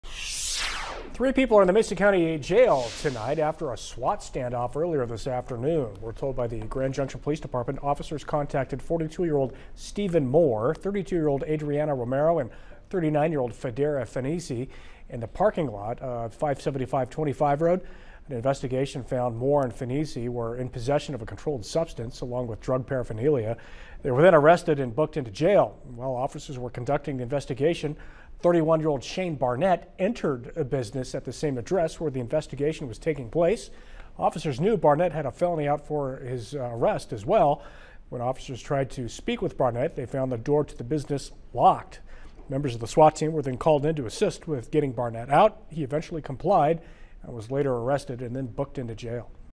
SWAT standoff leads to 3 people in Mesa County Detention Facility
Three people have been booked into the Mesa County Detention Facility after a SWAT standoff.